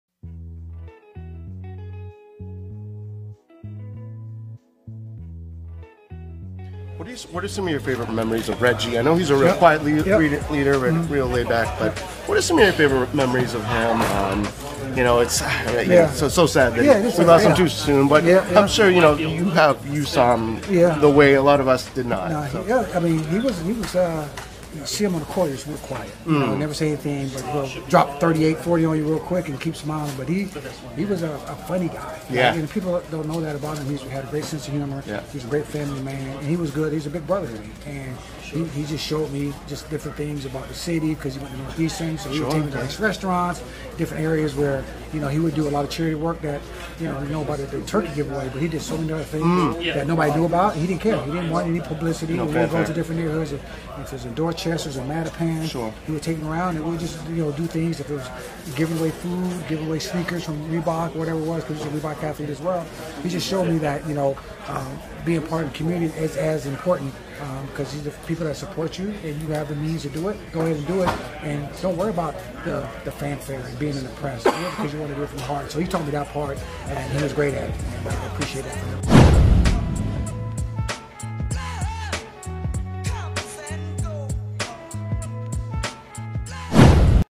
Meeting Boston Celtics Captain Reggie Lewis
Throwing it back to the first time, I met Reggie Lewis. It is a special memory to me, one that will be forever etched in my brain until the end of time. I was a super shy kid. Meeting Reggie back then was an overwhelming moment for me. I could not process the moment in real time. Here was my larger than life, almost mythical superhero figure right there in the flesh in front of me. I had so much to say, but I could not get a word out. I’m sure Reggie could tell how nervous I was and in a way that only Reggie could he was able to extend out his hand with that gentle compassion to try to make me feel at ease. I never got to say all that I wanted to say to him, but even without saying a word I think he could see how much he meant to me and there was just a silent calm between us, and understanding of what the momment meant for a wide eyes kid like me. It put me at ease and I'm forever greatful to him for that. (Shit who’s cutting up onions in here.) Man do I ever miss Reggie. Losing him is something I don’t think that I will ever truly get over but I’m so grateful that I had this one special moment with my hero. Last year, I had the honor of interviewing one of Reggie‘s closest teammates, Dee Brown. Take a listen to the beautiful things had to say about Reggie as he takes us down a trip of memory lane. RIP my friend! DifferentHere Celtics ReggieLewis Respect Hero OurBleepinCity BleedGreen @Celtics